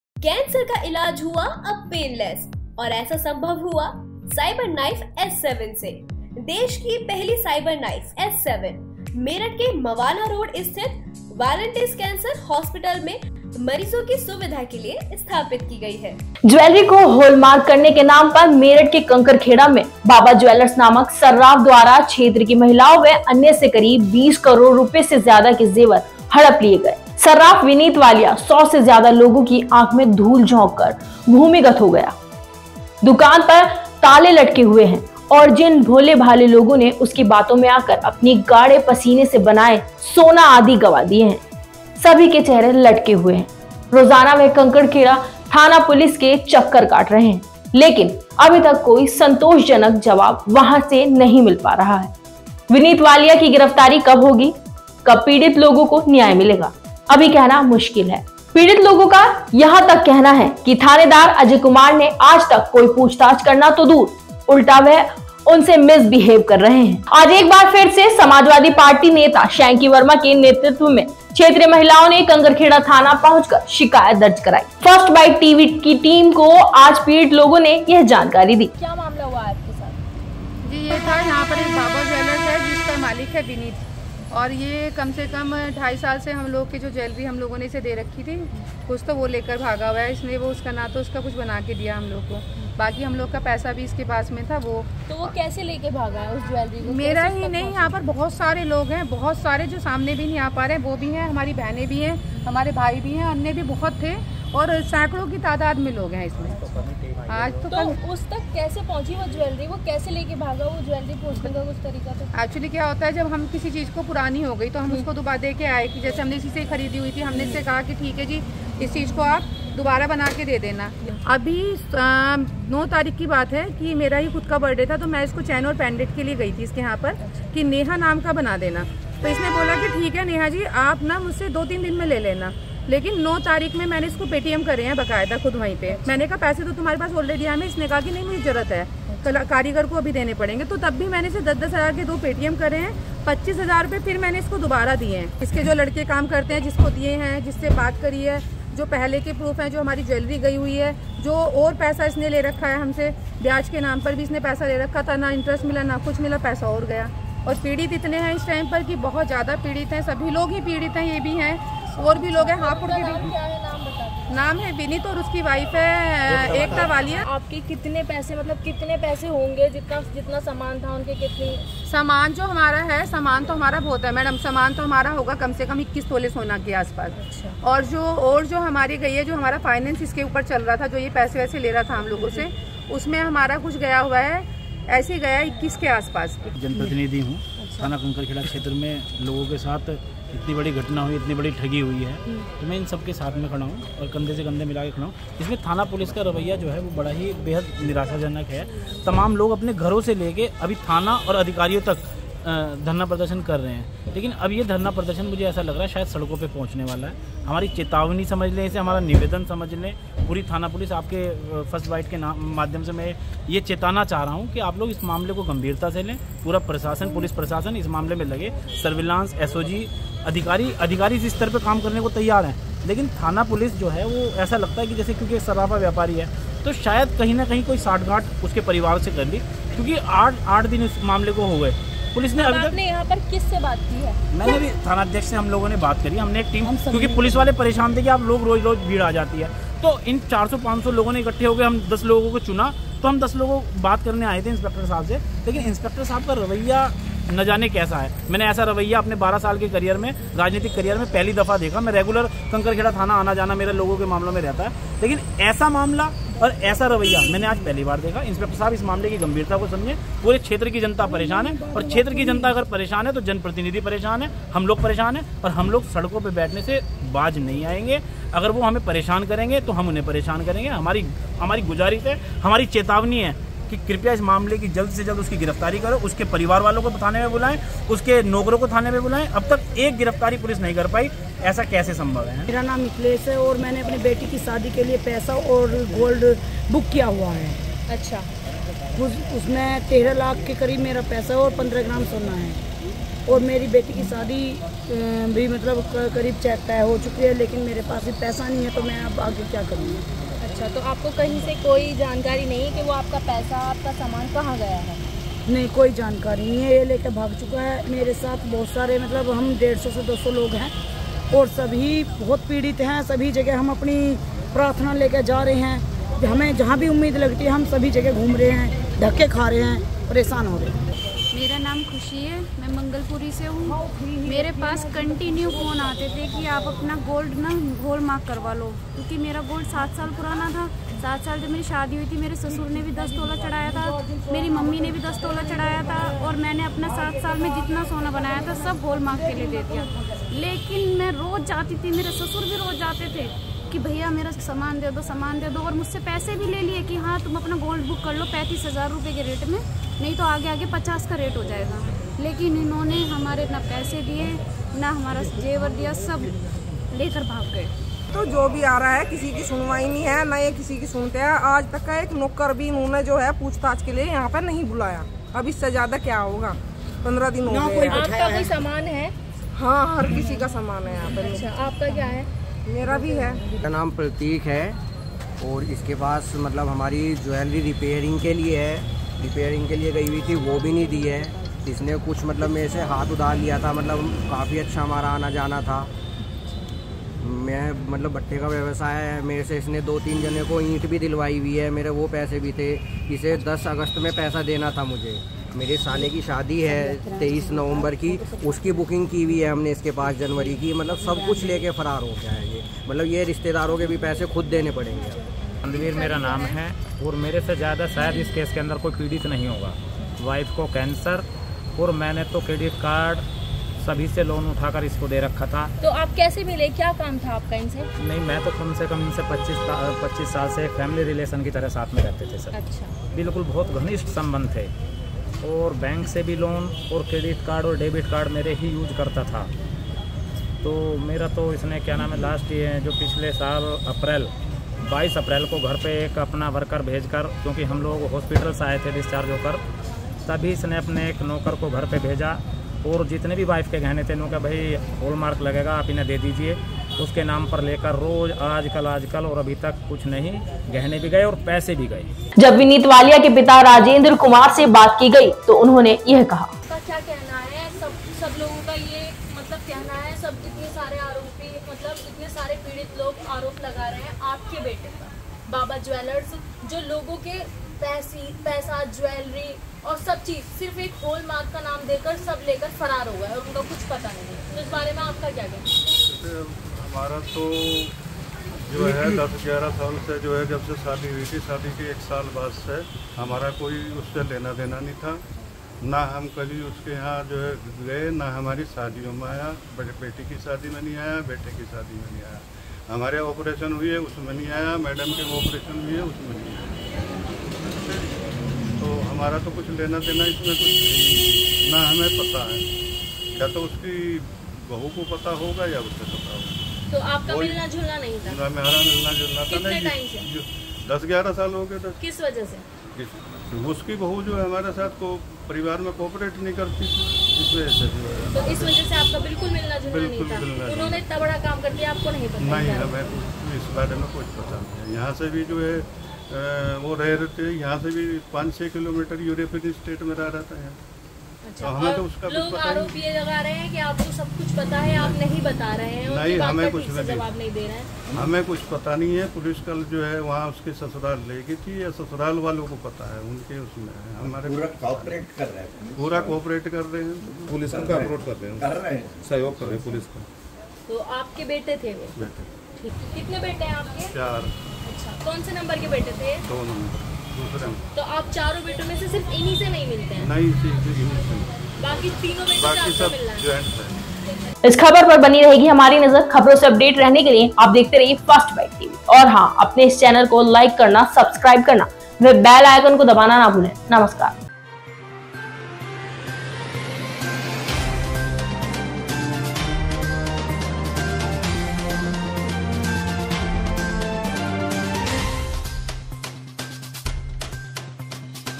कंकरखेड़ा में कोई बचा नहीं, जिसे सर्राफ विनीत वालिया ने ठगा नहीं । First Byte.Tv ने जानी व्यथा
हाॅल मार्क कराने के नाम पर ठगे जेवर
हॅाल मार्क न होने पर जेवर बेकार होने का दिया झांसा
महिलाओं को फोन कर अपनी बातों में फंसाया
विनीत वालिया के खिलाफ थाने में कई तहरीर
करीब दस से पंद्रह करोड़ का फटका बताया जा रहा
ज्वैलरी को होल मार्क करने के नाम पर मेरठ के कंकरखेड़ा में बाबा ज्वैलर्स नामक सर्राफ द्वारा क्षेत्र की महिलाओं व अन्य से करीब बीस करोड़ रूपये से ज्यादा के जेवर हड़प लिये गये। सर्राफ विनीत वालिया सौ से ज्यादा लोगों की आंख में धूल झोंक कर भूमिगत हो गया है। दुकान पर ताले लटके हुए हैं और जिन भोले भाले लोगों ने उसकी बातों में आकर अपनी गाढ़े पसीने से बनाये सोना आदि गंवा दिये हैं,सभी के चेहरे लटके हुए हैं। रोजाना वह कंकरखेड़ा थाना पुलिस के चक्कर काट  रहे हैं लेकिन अभी तक कोई संतोषजनक जवाब वहां से नहीं मिल पा रहा है। विनीत वालिया की गिरफ्तारी कब होगी, कब पीड़ित लोगों को न्याय मिलेगा...अभी कहना मुश्किल है। पीड़ित लोगों का यहां तक कहना है कि थानेदार अजय कुमार ने आज तक कोई पूछताछ करना तो दूर उल्टा वह उनसे मिस बिहेव कर रहे हैं।
In Kankarkheda, Meerut, in the name of marking the jewelery as a whole, jewelers worth more than twenty crore rupees were snatched from the women and others of the area by a moneylender named Baba Jewellers. Saraf Vineet Walia has gone underground by throwing dust in the eyes of more than 100 people. The faces of the victims are hanging on the lock of the shop. Everyday he is going round the Kankarkheda police station but is not getting any satisfactory answer.